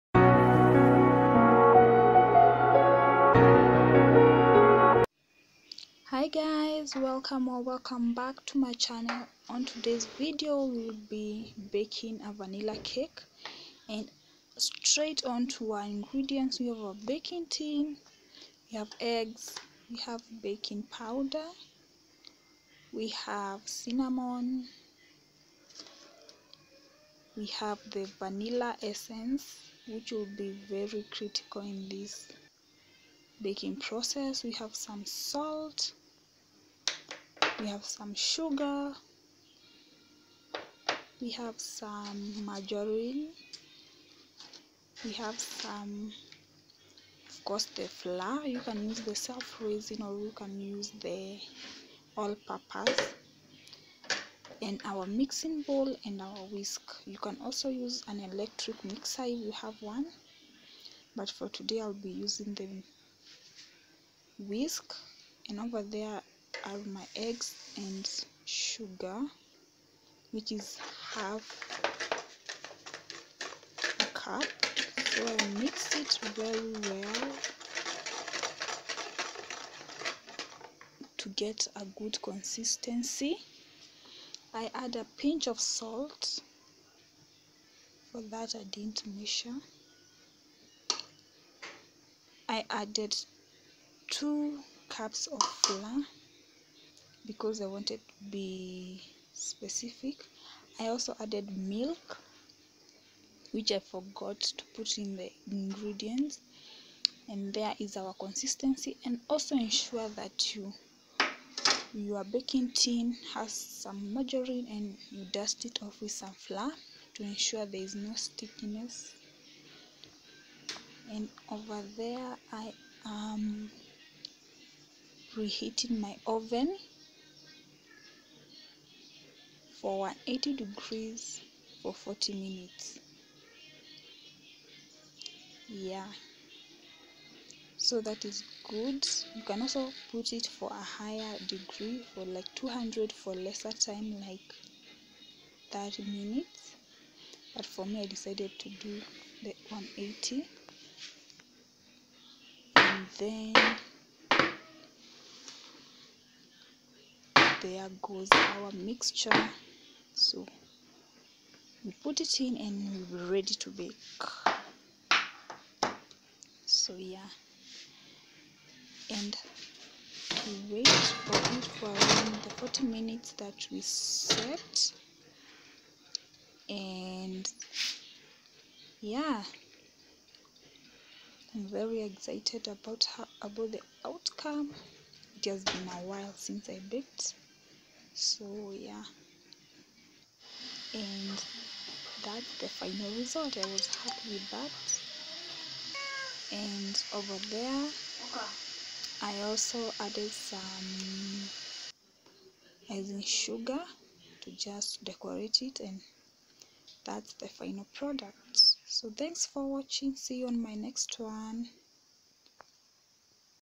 How to bake a vanilla-cinnamon cake the simplest way// Bake with me
Ingredients
1.Eggs
2.Cinnamon
3.Vanilla essence
4.Baking powder
5.Baking flour(all purpose or self raising)
6.Margerine
7.Sugar
8.salt